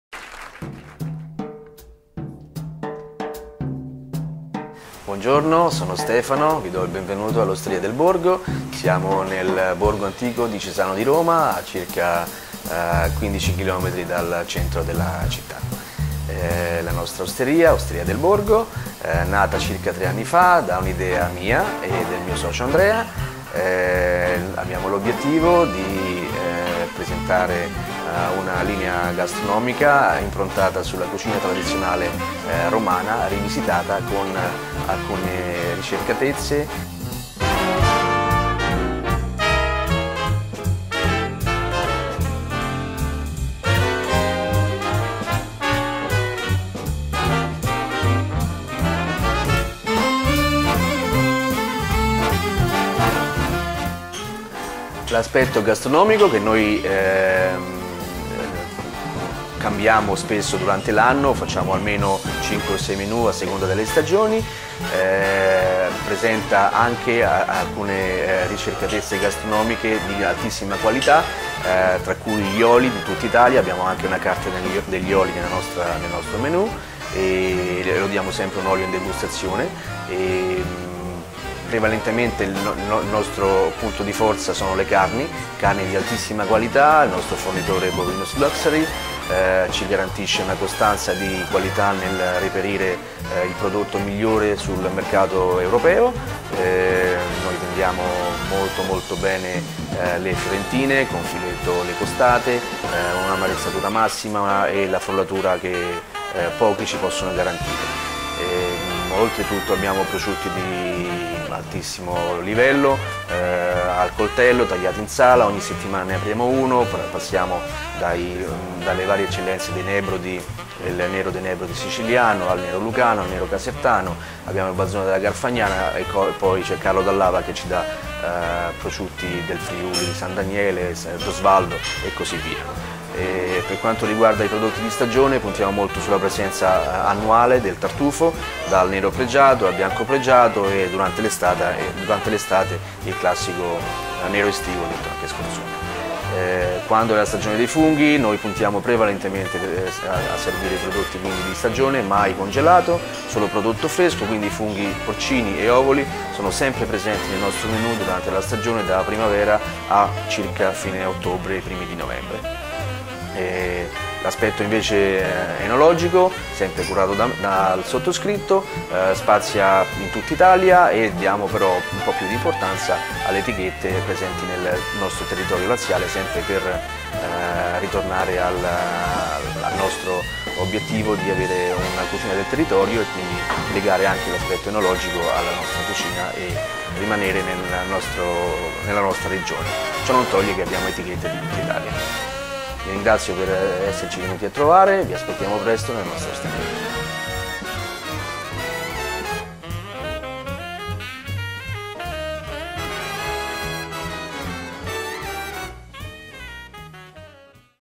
Osteria Del Borgo
Sapori veraci tipici della cucina laziale: cicoria ripassata, carciofi, carbonara, amatriciana, gnocchi, carni alla griglia e cinghiale in umido. Con un occhio alla stagionalità e alla filiera corta.